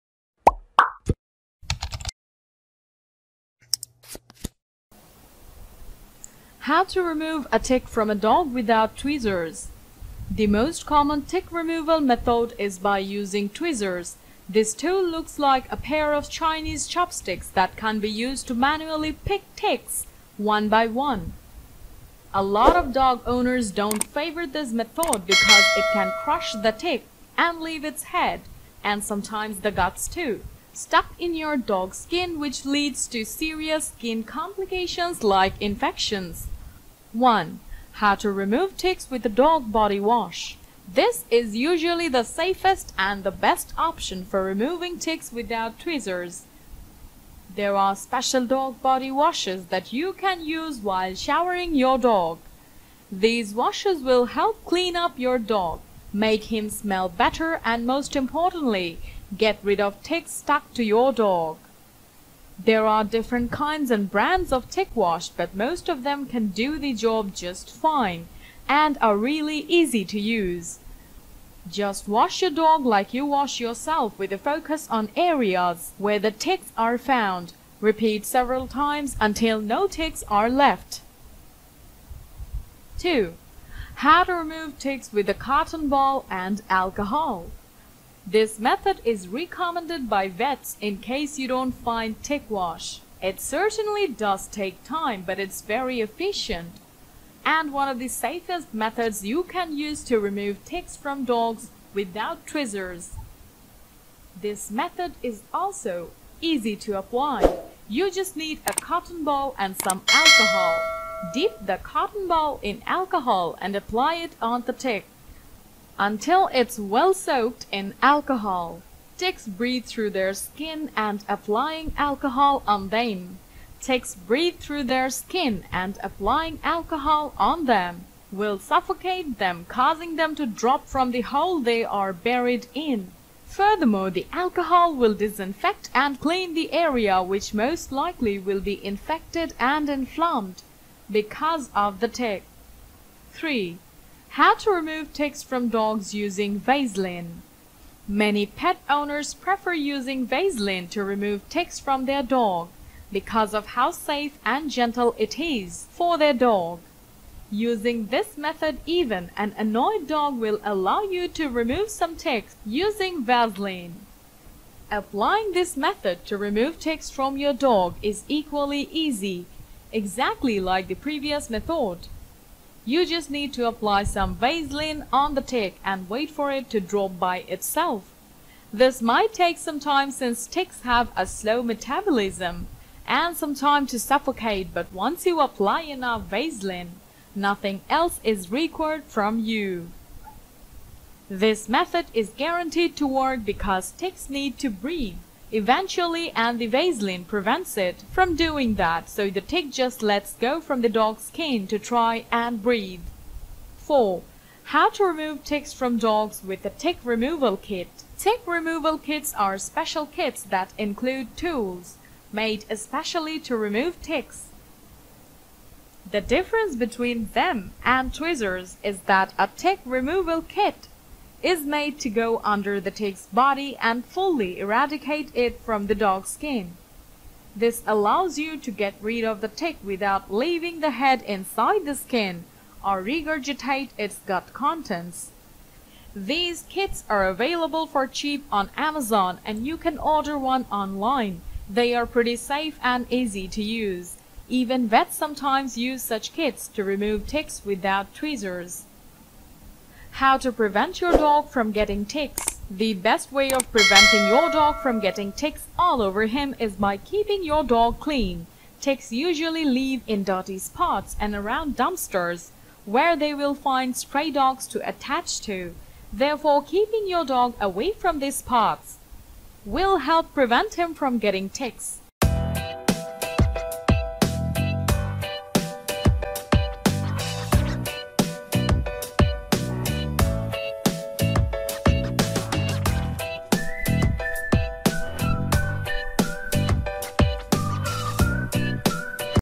How To Remove a Tick From a Dog Without Tweezers | Best Easy & Safe Ticks Removal Hacks
Are you worried about how to remove a tick from a dog without tweezers? The great solutions are shown in this video for preventing dog ticks at home.

Ticks are big enough for you to spot them, they seem like dark spots or lumps on your dog’s body. Sometimes they are tough to spot if your dog has a strong dark coat, however, you can brush the coat or run your hand through it to spot the ticks.

Here we have tried to mention and recommend some best safe and easy ways to get rid of ticks from your dog. The methods are in the below-
1. How to remove ticks with dog body wash?
2. How to remove ticks with a cotton ball and alcohol?
3. How to remove ticks from dogs using vaseline?
4. How to Remove Ticks from dogs with a tick-removal kit?